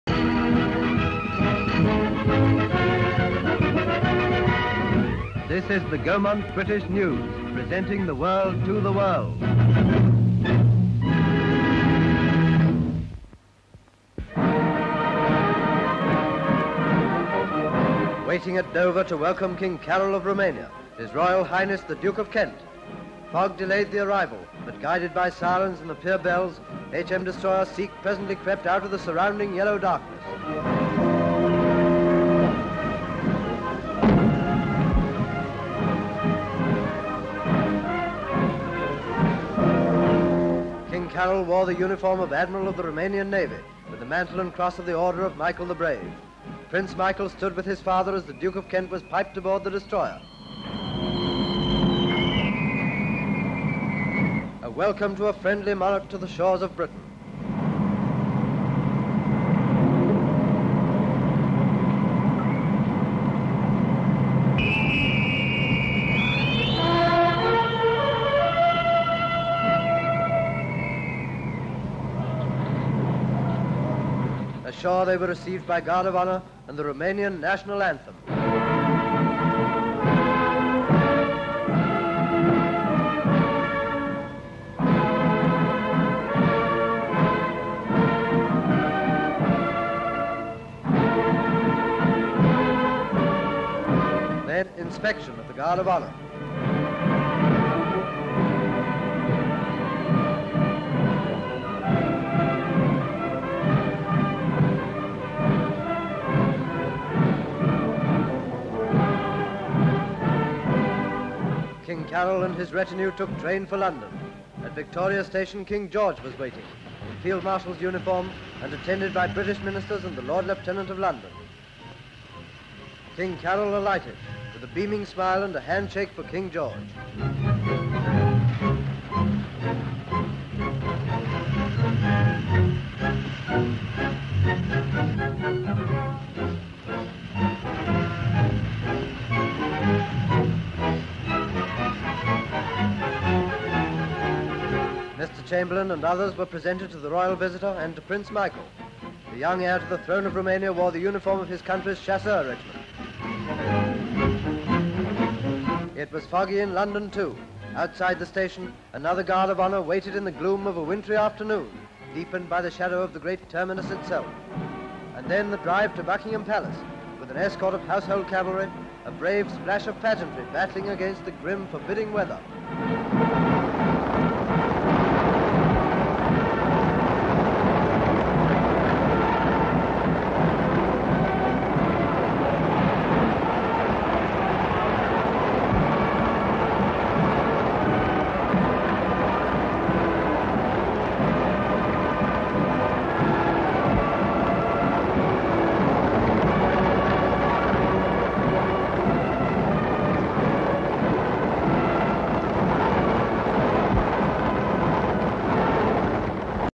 King Carol of Romania arrives in England (1938)
King Carol of Romania and Prince Michael arrive in England and are greeted by King George VI and the Duke of Kent

SLATE INFORMATION: King Carol Visits Great Britain

ENGLAND: Dover:
EXT

CAROL, King of Rumania (Deceased) Arrives w. Prince Michael in England.

CHAMBERLAIN Shakes hands with King Carol in London

GEORGE VI Greet King Carol at Victoria Station.

KENT, Duke of Greets King Carol at Dover.

MICHAEL/ CROWN Prince of Rumania Arrives w. King Carol in England, & drives to the City

SIKH, H.M.S. Destroyer v.g. shot of arrival in fog w. King Carol.

King Carol II of Romania (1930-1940), Royalty

Background: King Carol of Romania and Prince Michael arrive in England and are greeted by King George VI and the Duke of Kent